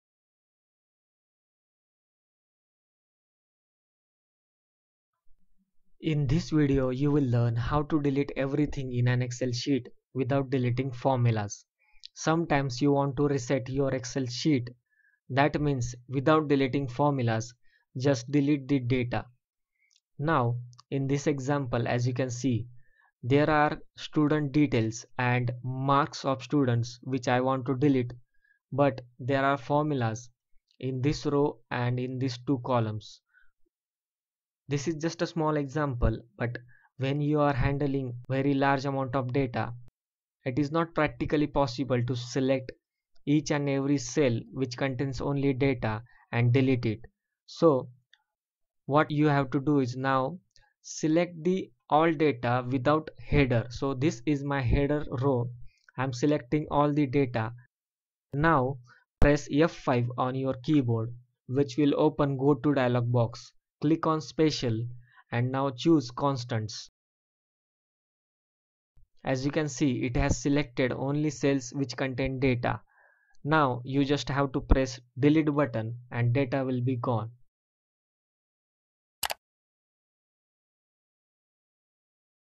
Delete All Data in a sheet but Keep Formulas | Excel
Delete your data but not your formulas. Sometimes you have to delete all the data that was entered into Excel sheet while being careful not to delete any of your formulas.

Watch this video to learn how to delete data in excel and not formulas.

Excel_Series is comprehensive video series includes Microsoft Excel tutorial. You will find basic to advanced level Excel training videos for free here!